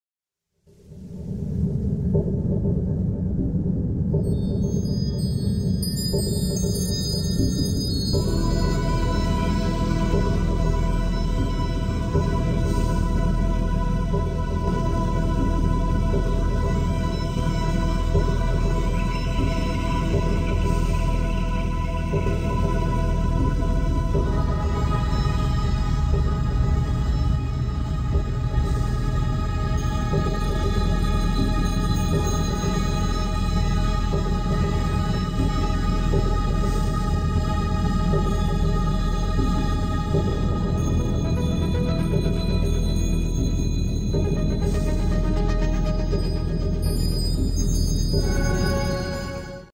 Sistema Solar
Sample of an animated short, made for the Artist Sandra Ramos, made by: Yoel Alegre Lago (HappYoe) modeling animation and music: Yoel Alegre Lago#animation